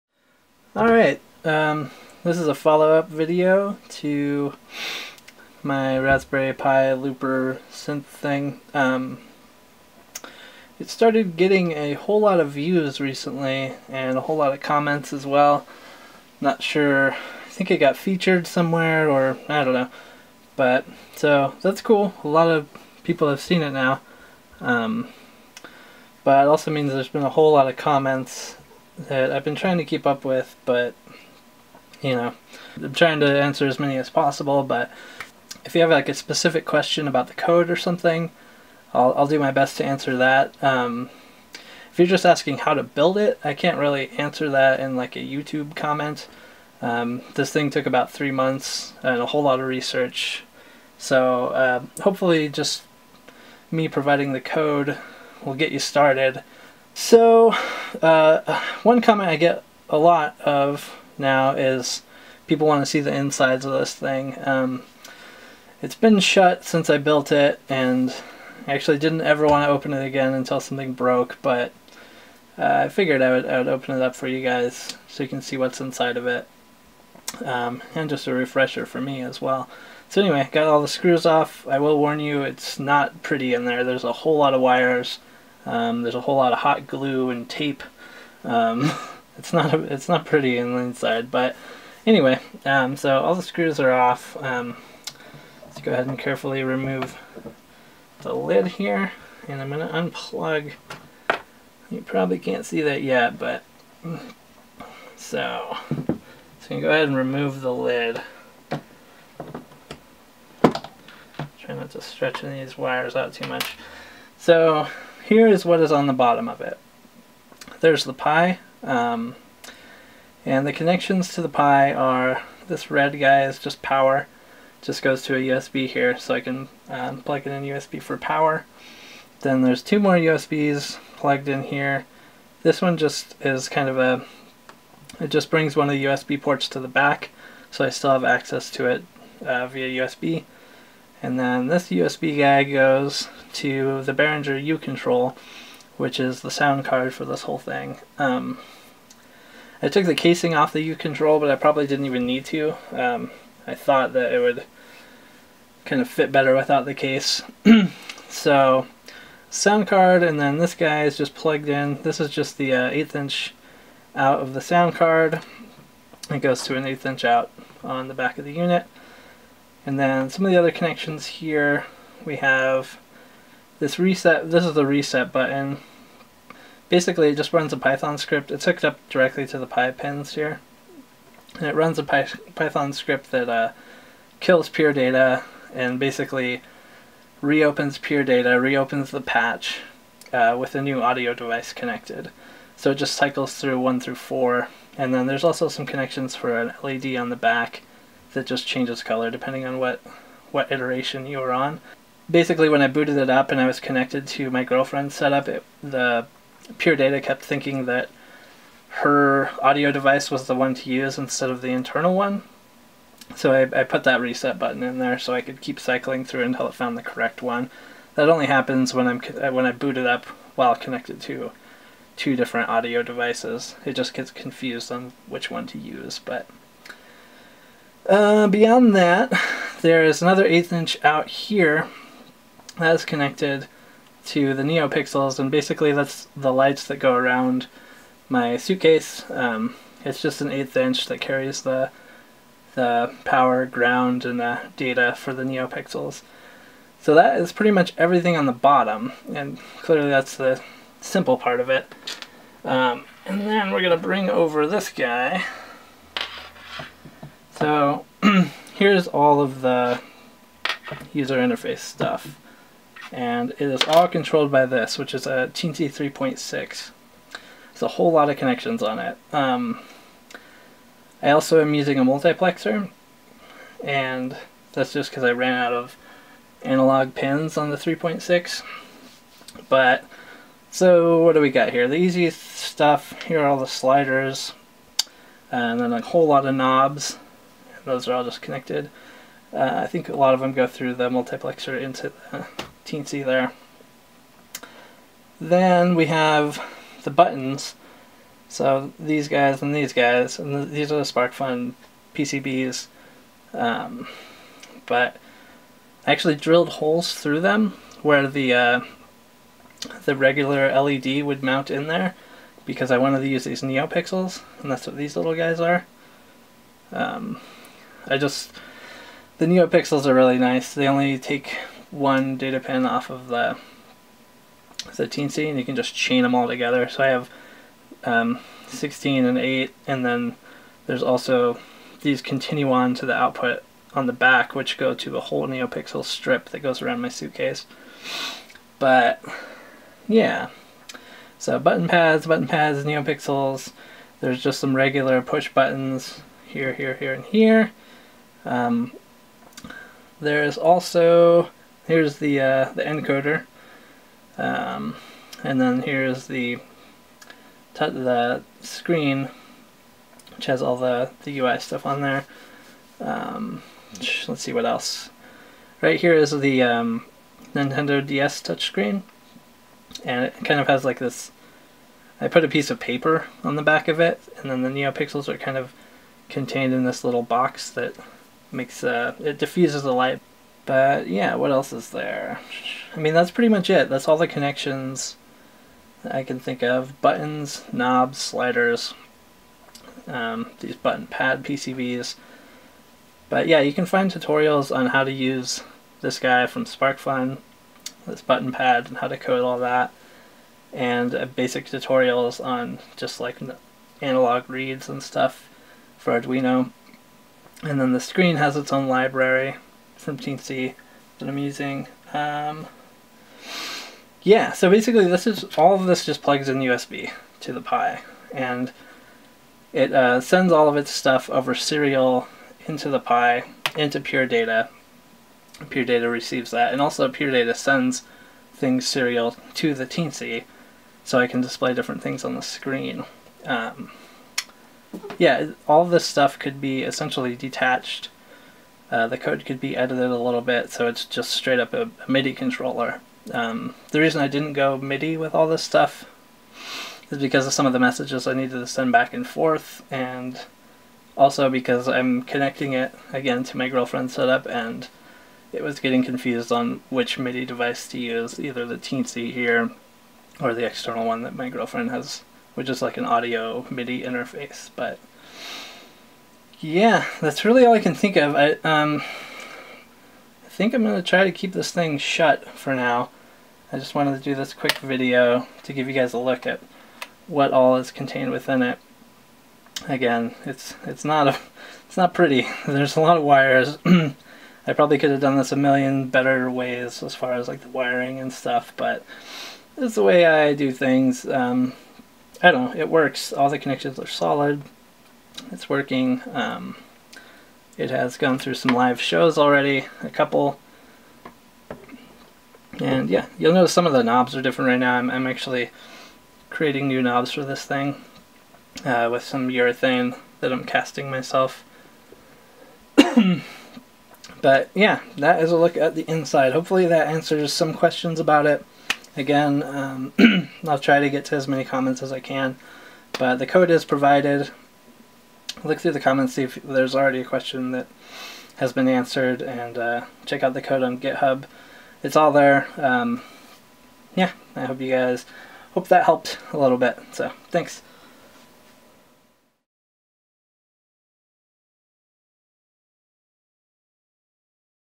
Raspberry Pi Looper Guts
Thought I'd do a quick overview of the guts of my pi project. Seems like many folks have been interested in seeing what the internals look like.